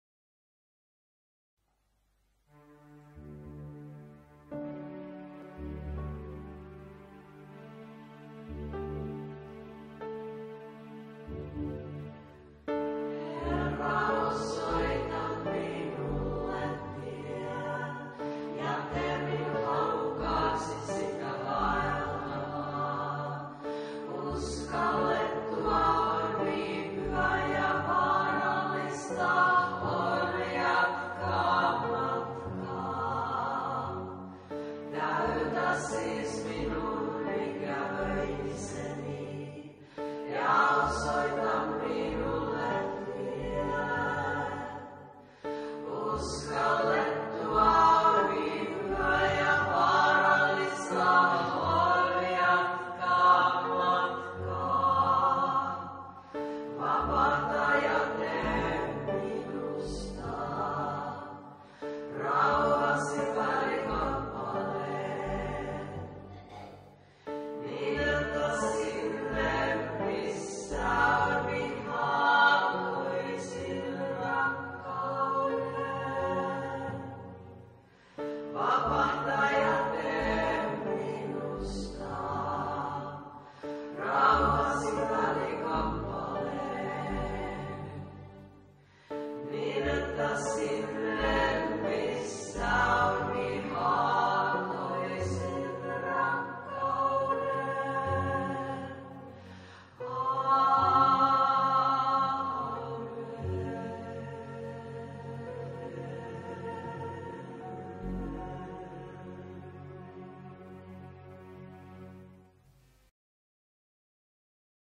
Rukous 19032022 Holopainen
Ensimmäinen rukous on Pyhän Birgitan, toinen on Franciscus Assisilaisen